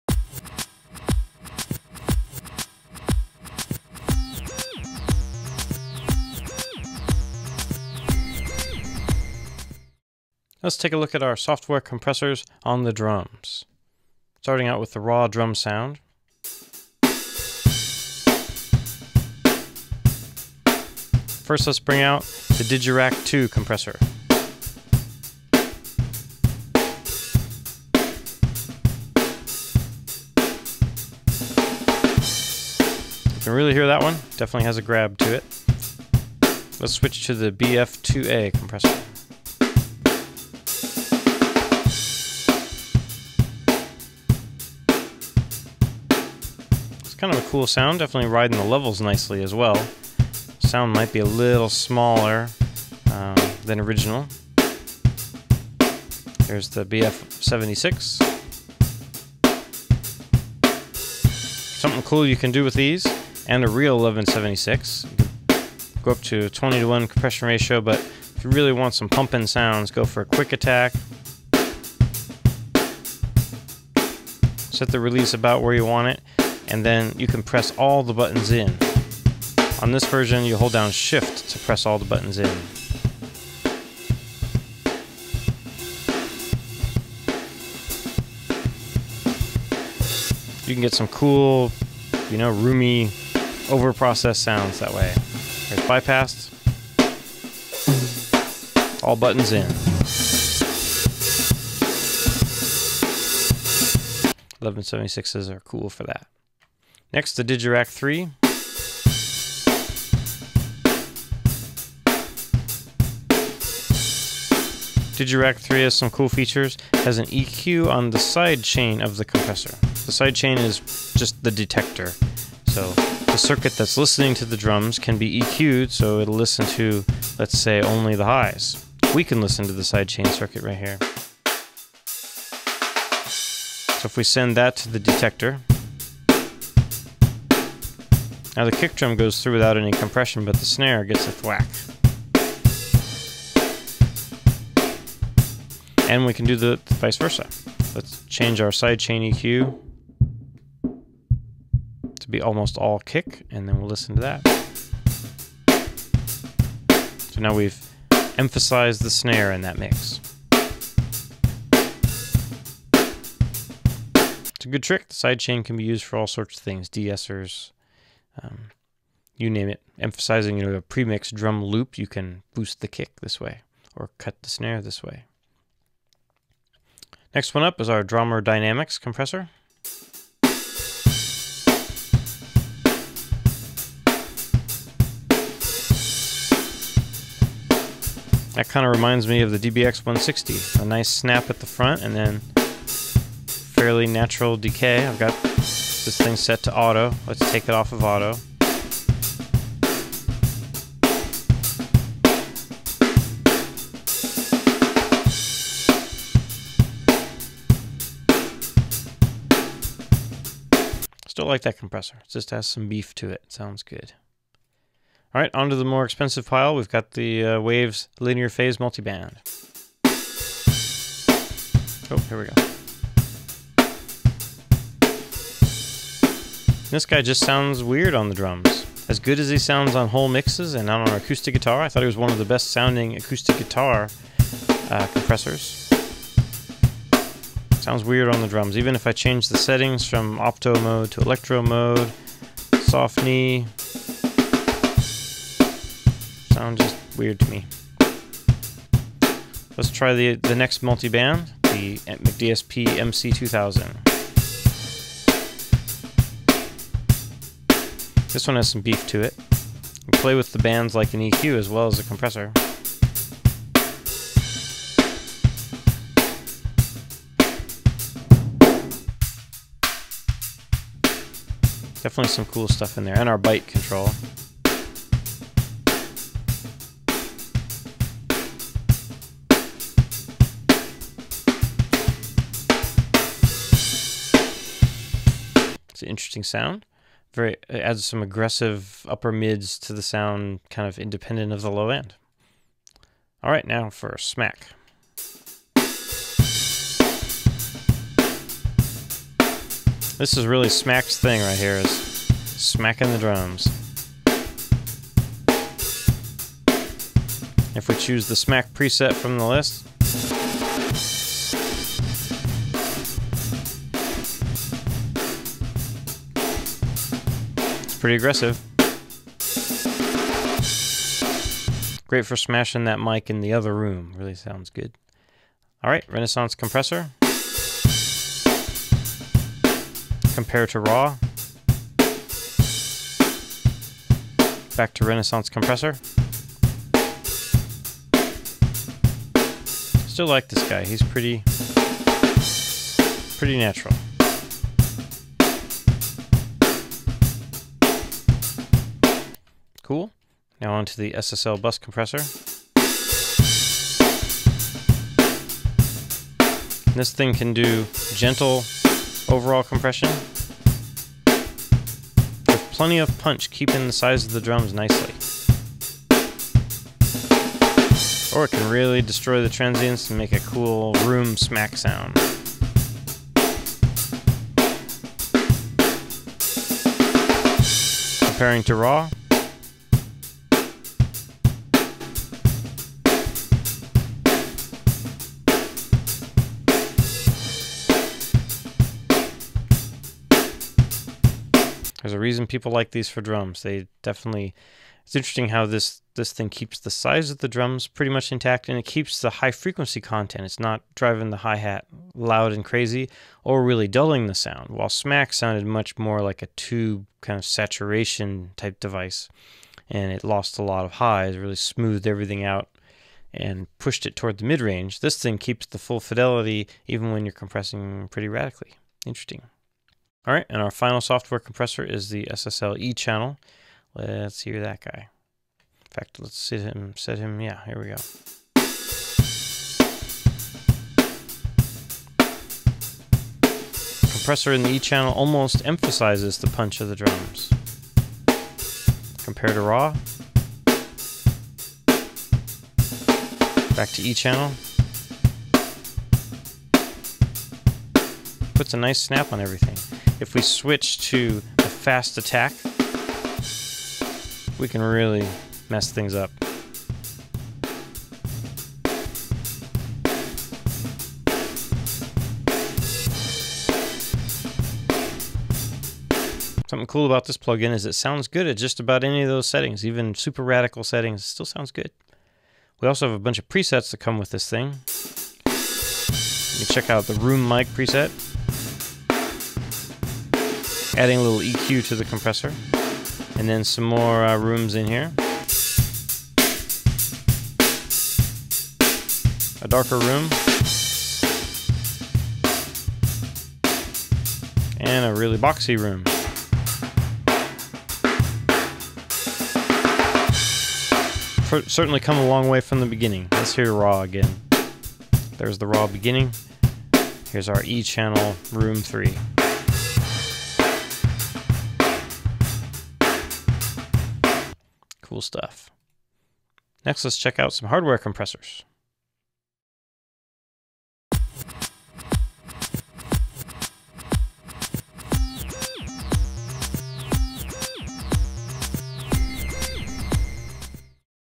Comparing Compressors on Drums
Taken from the "Compression: The Essentials" tutorial by HowAudio.com, audio engineer Jeff Dykhouse compares the Digirack II, Bomb Factory LA-2A, Digirack III, Drawmer Dynamics, Waves Linear Phase Multi-band, Smack, Renaissance, and Waves SSL E Channel compressors on a drum track.  You to listen to each and identify the differences.

If you are interested in learning more about compression and hearing the strengths and weaknesses of different software and hardware compressors,

Compressors included in the tutorial are:

Software:
1.  Digirack Compressor II
2.  Bomb Factory 1176
3.  Drawmer GCL
4.  Waves SSL E Channel
5.  Waves Master Bus Compressor
6.  Waves Linear Multi-band
7.  Waves  Renaissance  Compressor
8.  Bomb Factory LA-2A
9.  McDSP MC2000 Multi-band
10.  Digirack Compressor III
11.  Digidesign Smack!

Hardware:
1.  Teletronix LA-3A
2.  Smart Research C2
3.  dbx 160x
4.  Aphex Compellor
5.  Behringer  Tube Composer T1952